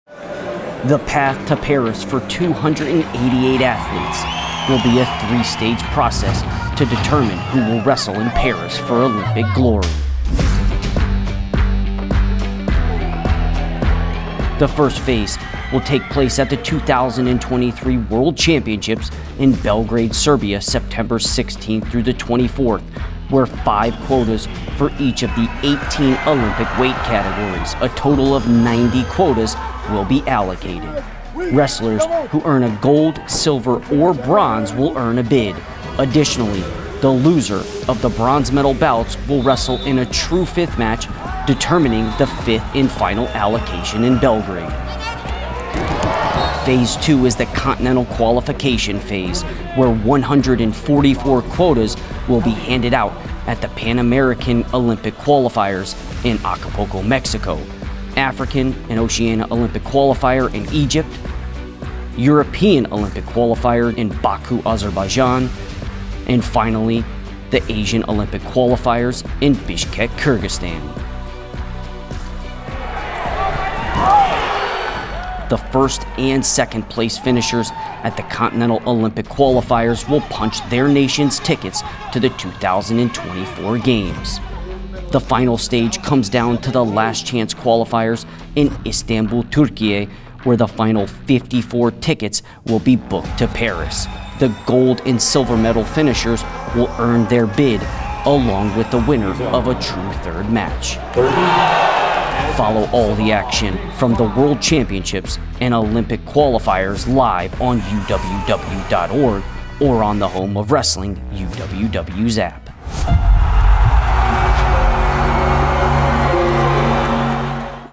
How do wrestlers qualify for the Paris Olympic Games?
- A new era of following wrestling with a subscription based video platform built for wrestling fans around the world!

The path to Paris for 288 athletes will be a three stage process to determine who will wrestle for Olympic glory.

The first phase will take place at the 2023 World Championships in in Belgrade, Serbia, September 16-24th, where five quotas for the each of the 18 Olympic categories—a total of 90 quotas—will be allocated.

Wrestlers who earn a gold, silver or bronze will earn a bid. Additionally, the losers of the bronze-medal matches will wrestle in true-fifth match, determining the fifth and final allocation from Belgrade.

Phase two is the continental qualification phase where 144 quotas handed out at the:
- Pan American Olympic Qualifier (Acapulco, Mexico, March 1-3)
- African Olympic Qualifier (Alexandria, Egypt, March 22-24)
- European Olympic Qualifier (Baku, Azerbaijan, April 4-7)
- Asian Olympic Qualifier (Bishkek, Kyrgyzstan, April 19-21)

The first and second place finishers at the Continental Olympic Qualifier earn an automatic bid to the Paris Olympics for their nation.

The final stage for the Paris Olympic qualifying process comes down to Istanbul, Türkiye (May 9-12), for the Last Chance Qualifier, where the final 54 tickets will be booked. The gold and silver medalists, and the winner of a true third match, will punch their ticket to the Olympics.

Follow all the action from the World Championships, Continental Olympic Qualifiers and the Last Chance Qualifier, live on UWW+ or on, “The Home Of Wrestling,” UWW’s app.